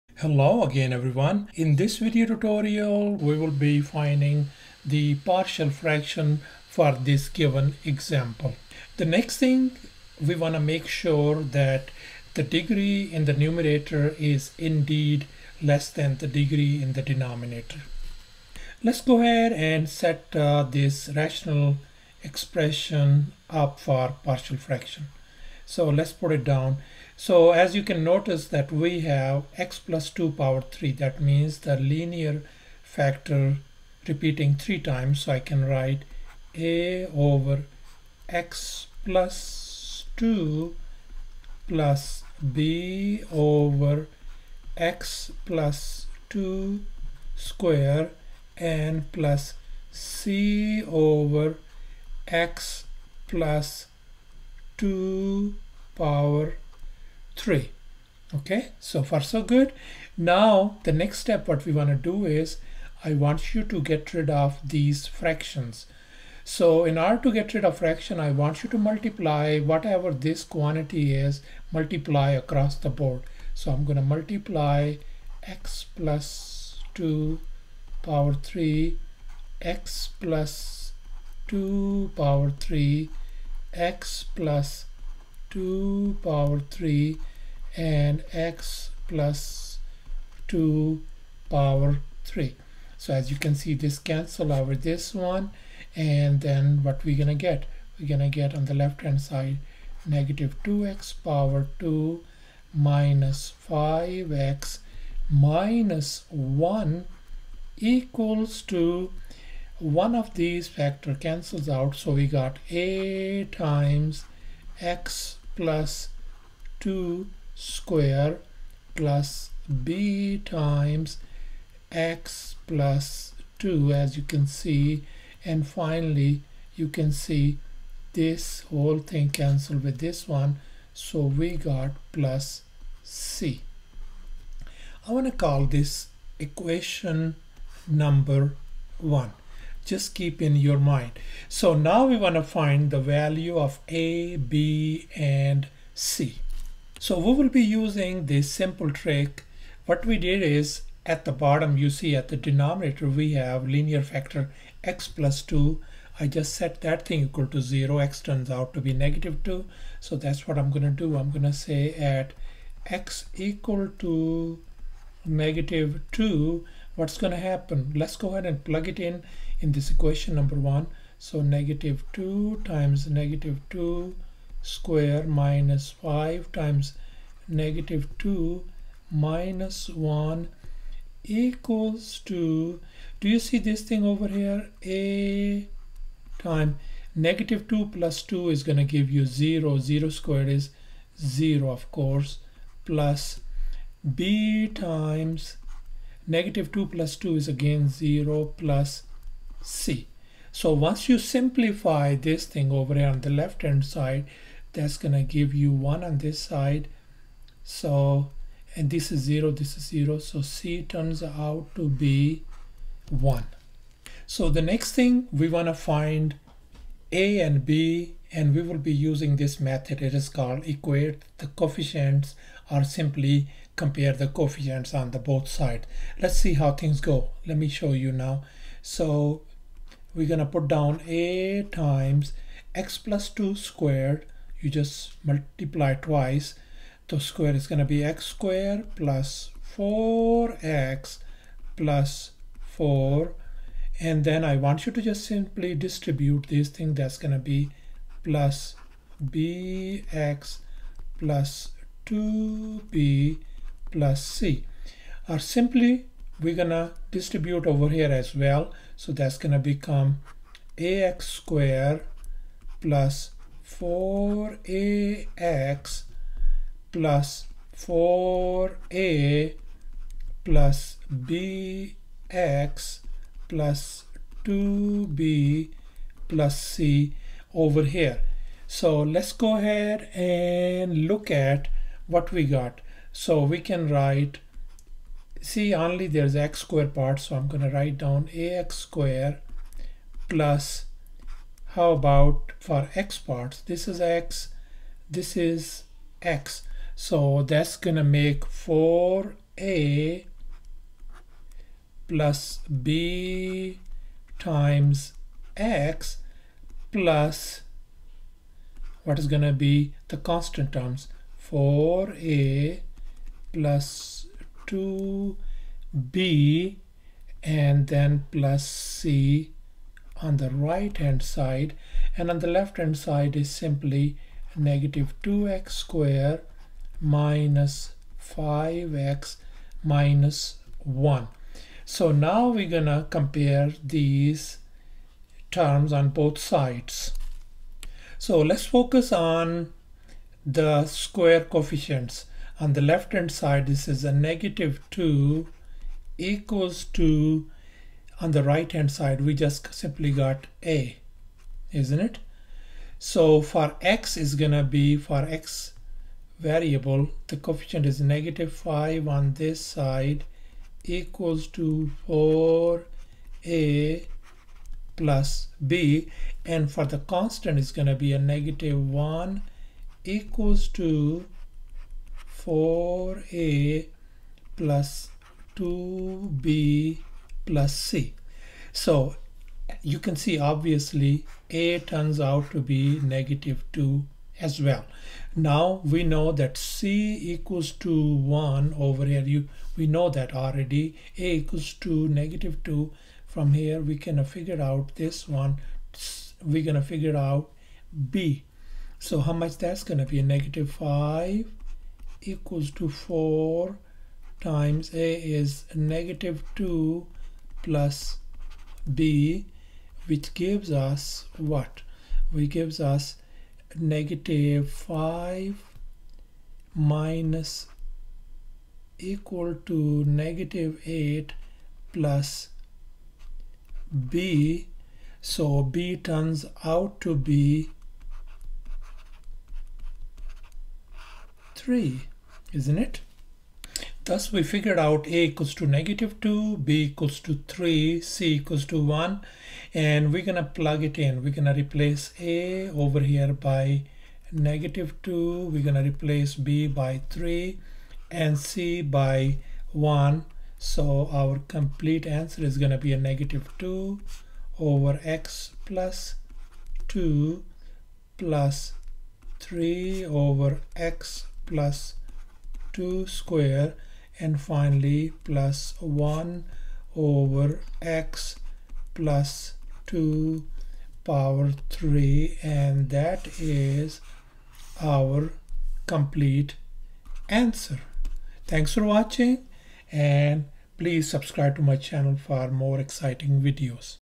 Find the Partial Fraction Decomposition - Step-By-Step Technique
Learn how to find the Partial Fraction Decomposition using this step-by-step technique. By PreMath.com